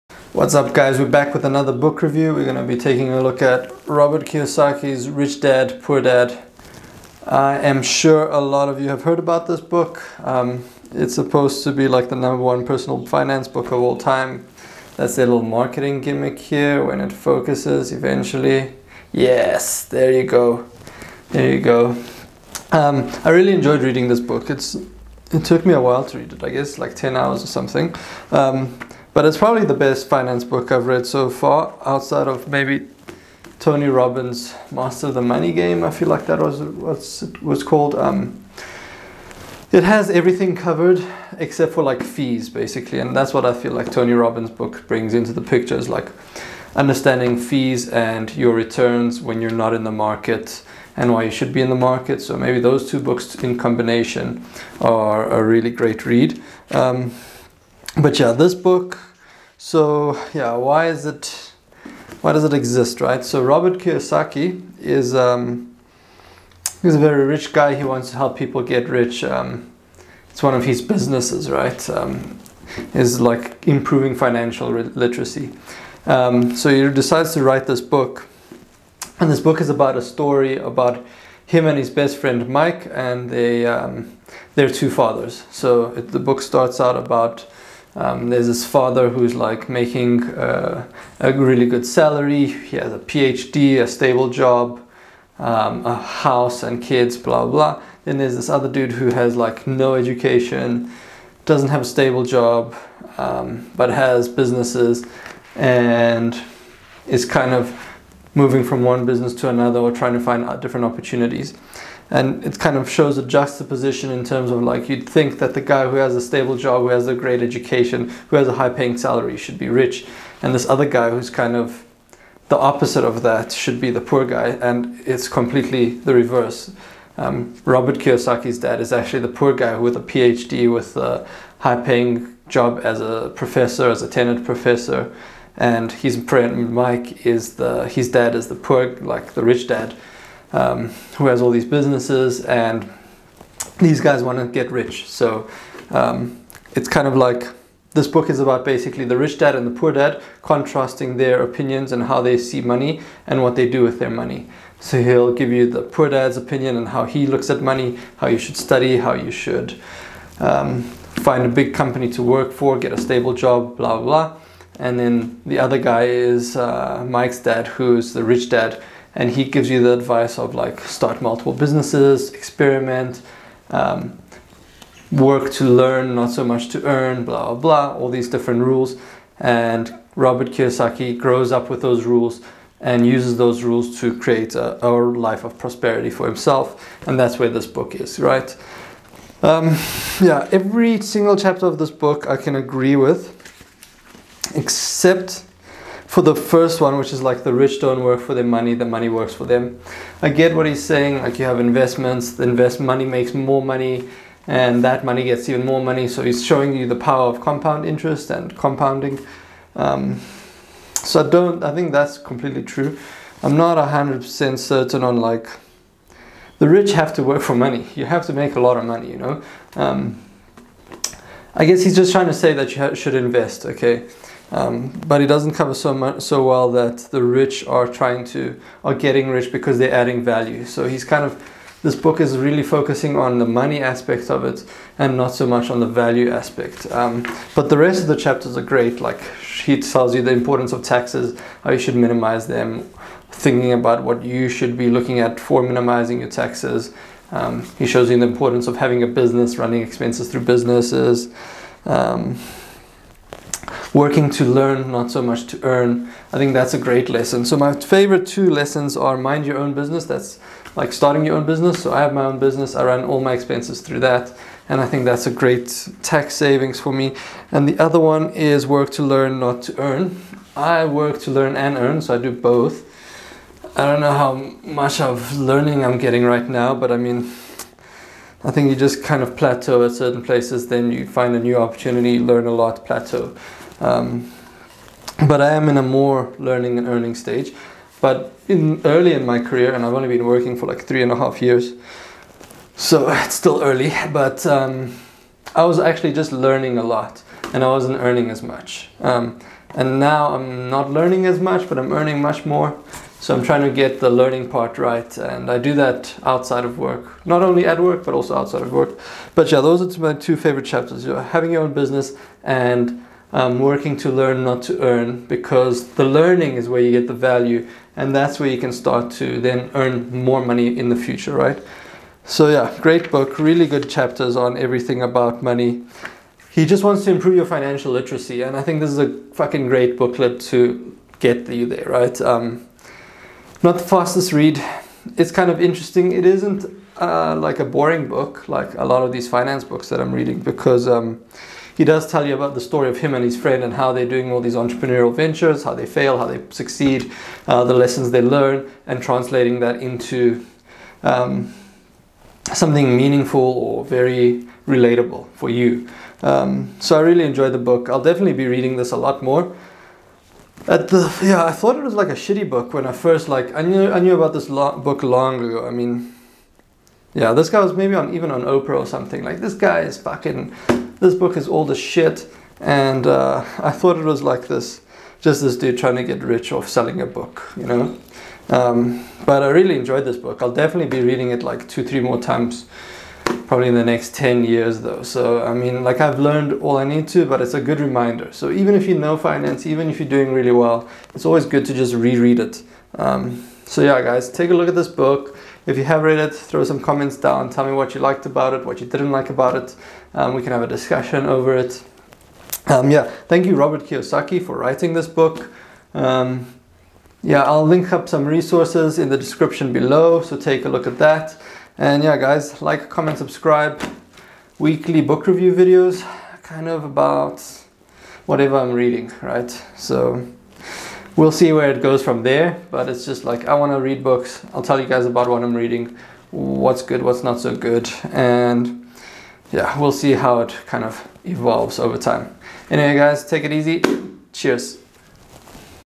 Robert Kiyosaki - Rich Dad Poor Dad
My two favorite chapters are Mind Your Own Business and Work To Learn, they highlight the importance of owning your own business to run expenses through PRE tax earnings, and how important it is to price learning and education at work, i.e. people focus on money and not the value of the education they are getting.